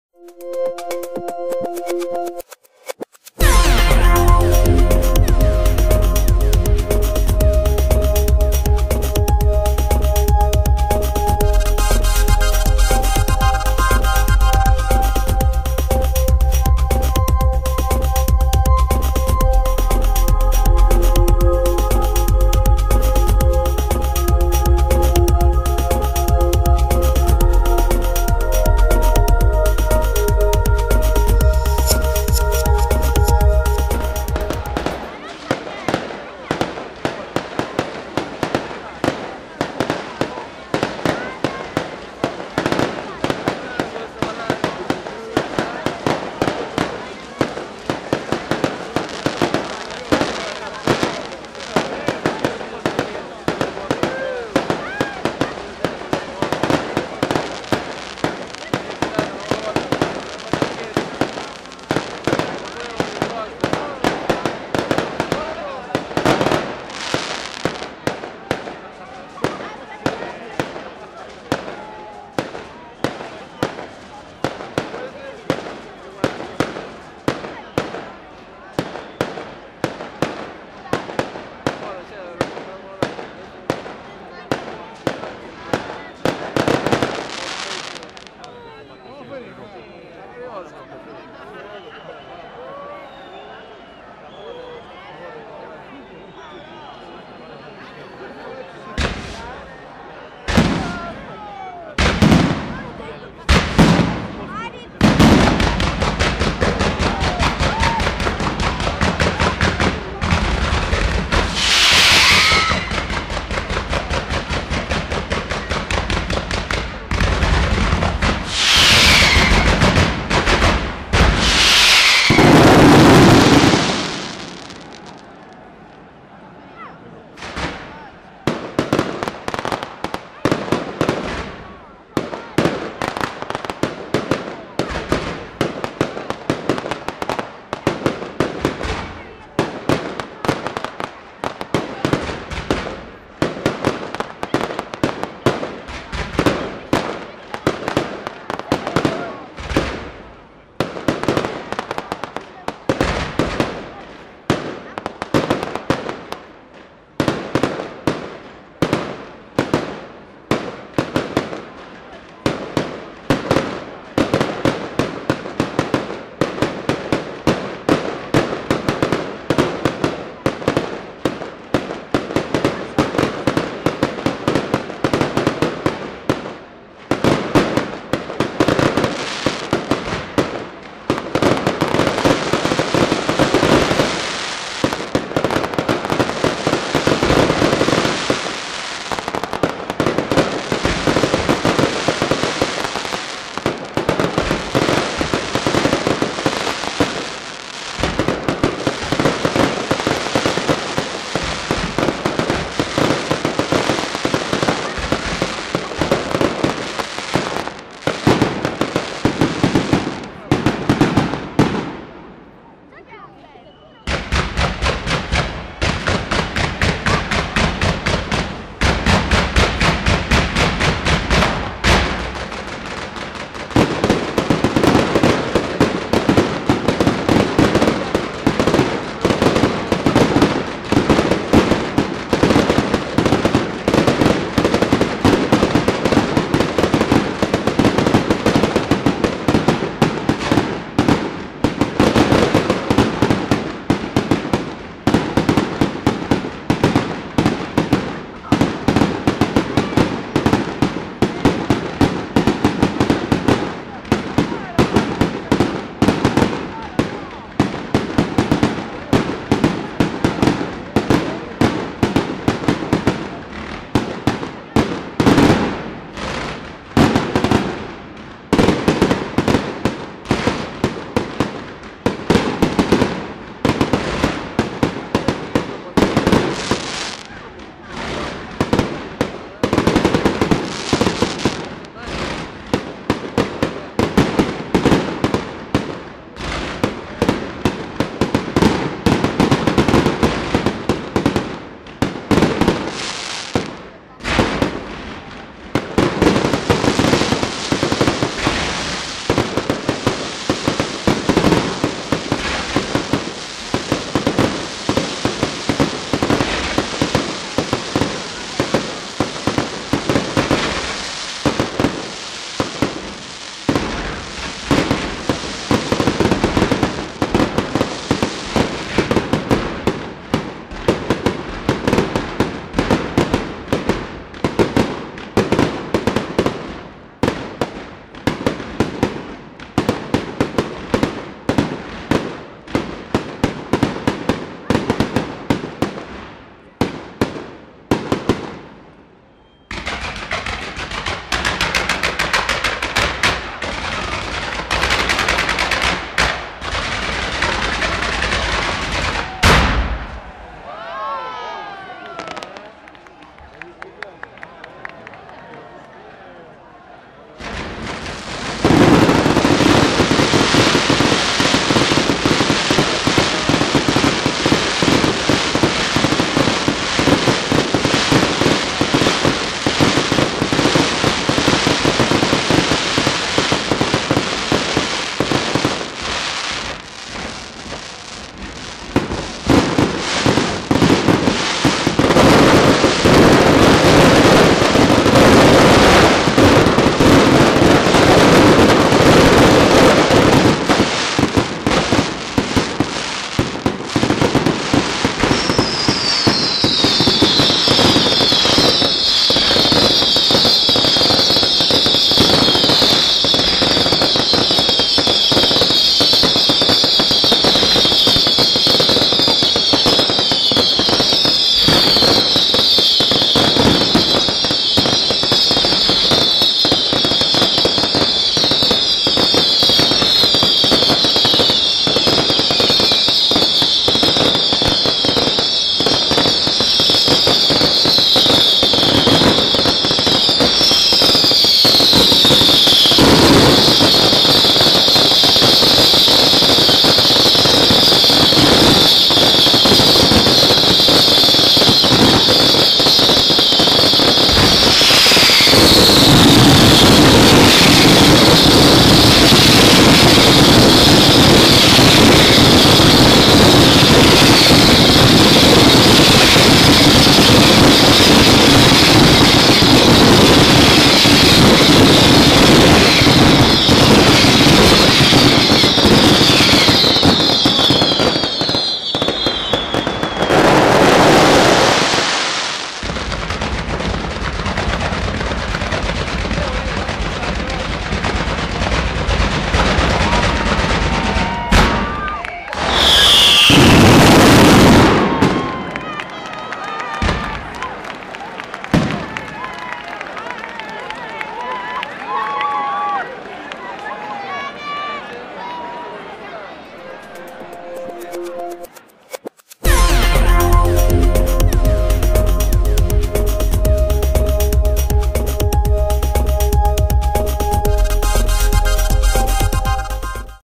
VIAGRANDE (Catania) - FIRESUD (2016)
VIAGRANDE (Catania) - 14 Gennaio 2016
Festeggiamenti in onore di San Mauro Abate

Spettacolo pirotecnico per il Quartiere Scalatelli
FIRESUD di Pappalardo e Leone da Zafferana Etnea (Ct)